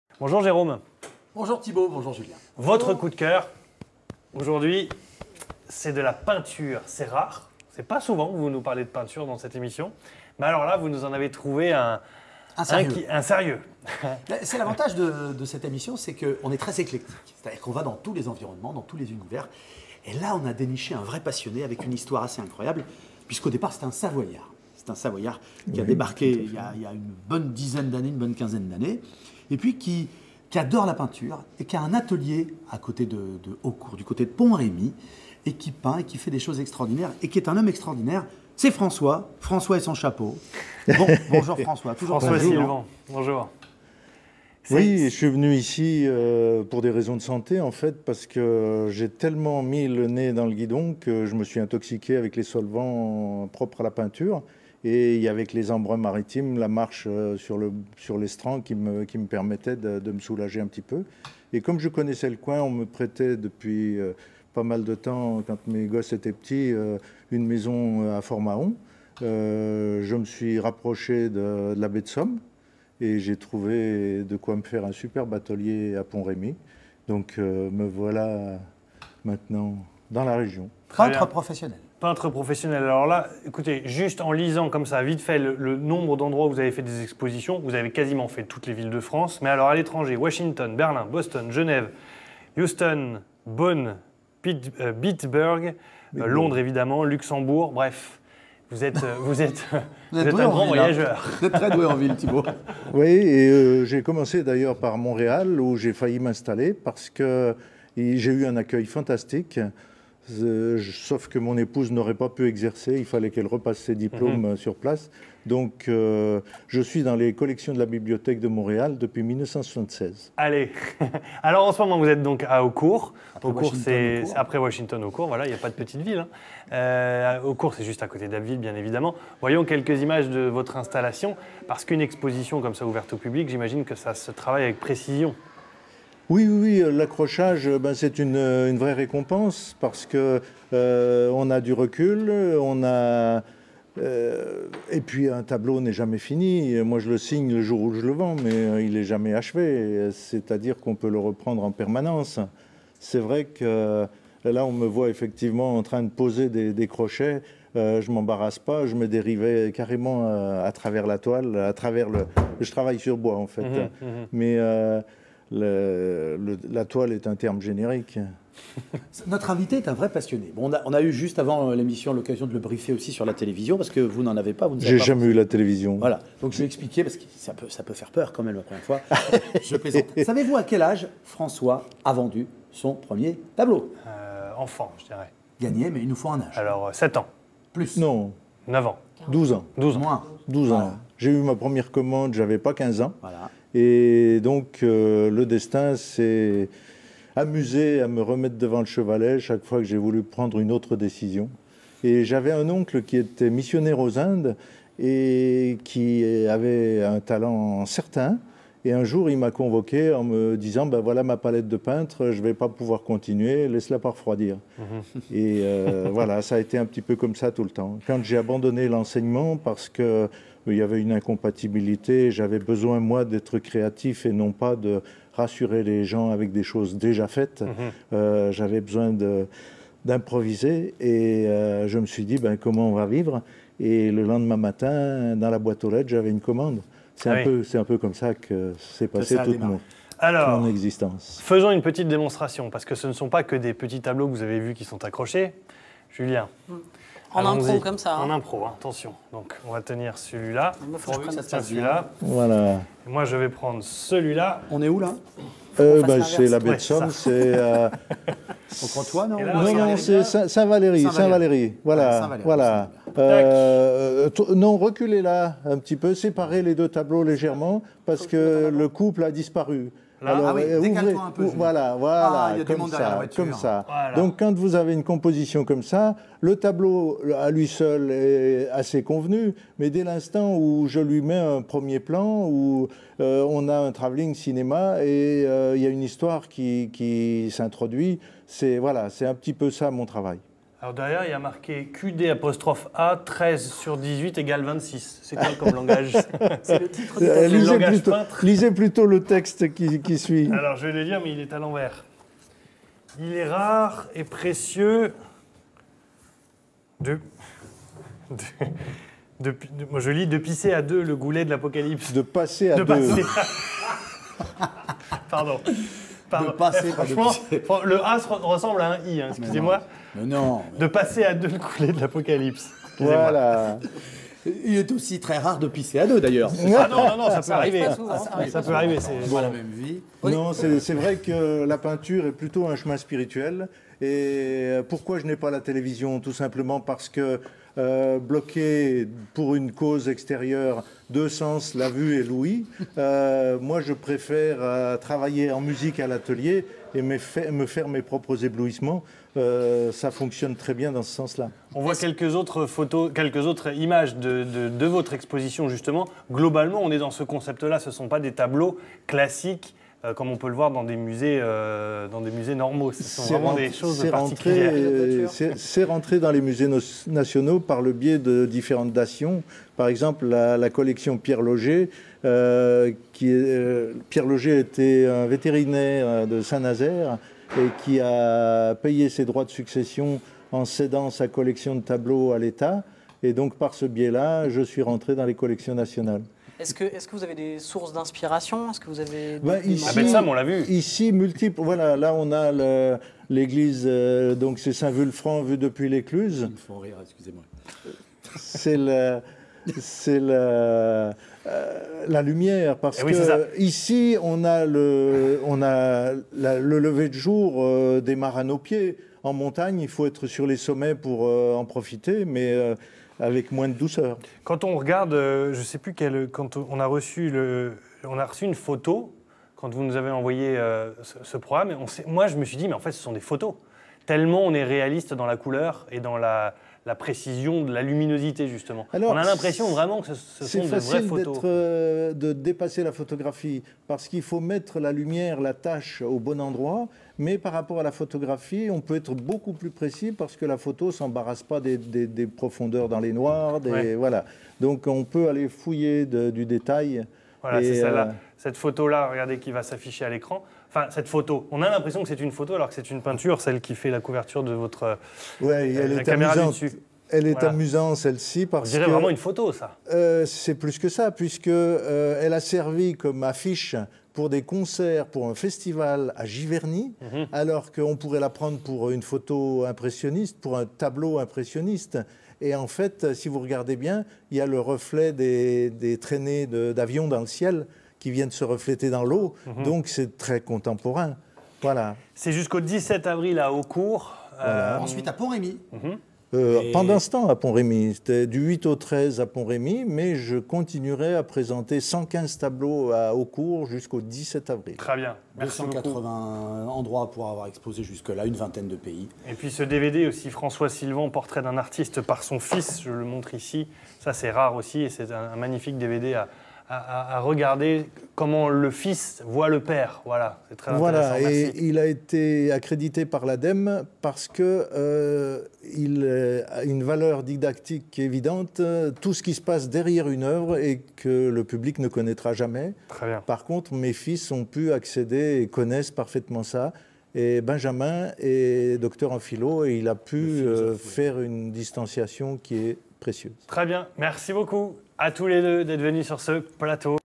La peinture de François Sylvand : la lumière, l'espace et le temps
Installé depuis une quinzaine d'années en Picardie, à Eaucourt dans la Somme, le peintre François Sylvand est, cette semaine, le coup de cœur de Jérome Jean. Un artiste qui a exposé dans de nombreuses villes de France mais aussi à Washington, Berlin, Londres ou Montréal. Il vient sur le plateau nous parler de cette passion pour la peinture qui l'anime depuis plus de 40 ans. Une de ses sources d'inspiration : La Baie de Somme et ses lumières